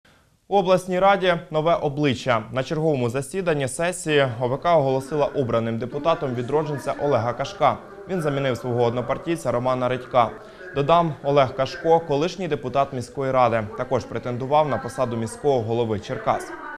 У Черкаській облраді – поповнення
На черговому засіданні сесії ОВК оголосила обраним депутатом «відродженця» Олега Кашка